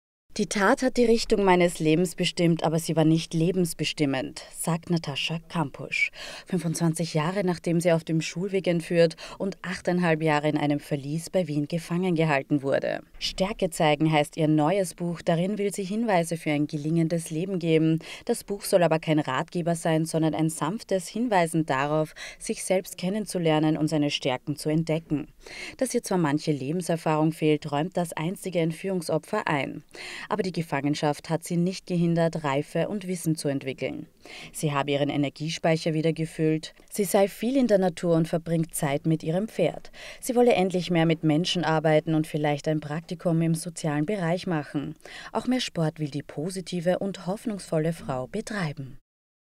25 Jahre nach Entführung: Kampusch zeigt Stärke – und freut sich über ihr Pferd
3096 Tage lang war sie in den Händen ihres Entführers – 25 Jahre danach kämpft Natascha Kampusch (35) immer noch um Normalität in ihrem Leben. Mit ihrem mittlerweile vierten Buch will sie sich und ihren Lesern dabei helfen, sich selbst kennenzulernen.